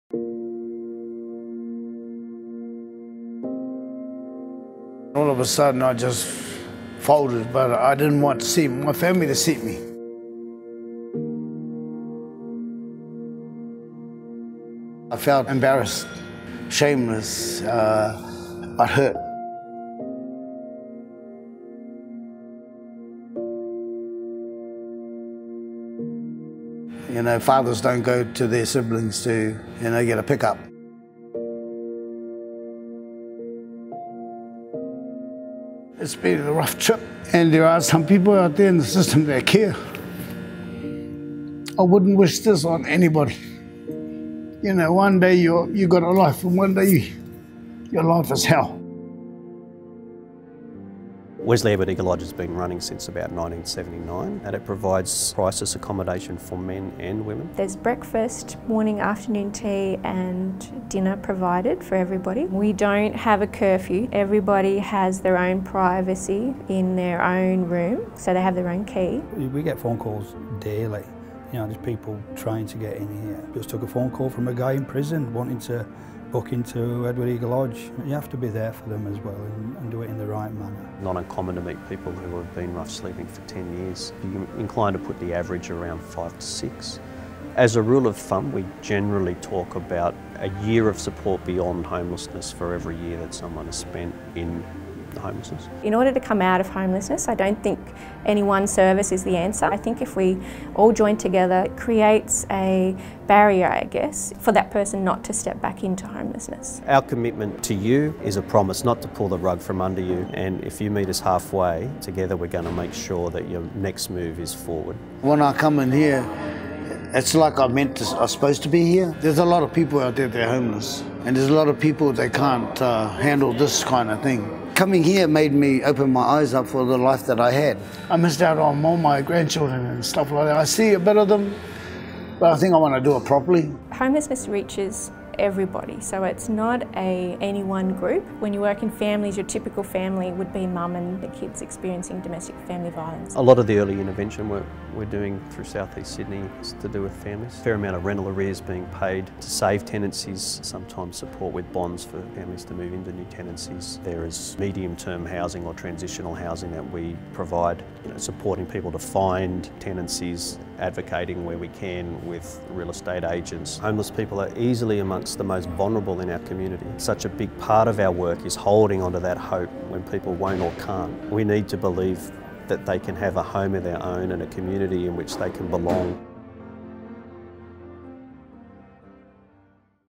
Wesley Mission: Homelessness and families
Did you know there are over 100,000 people experiencing homelessness in Australia?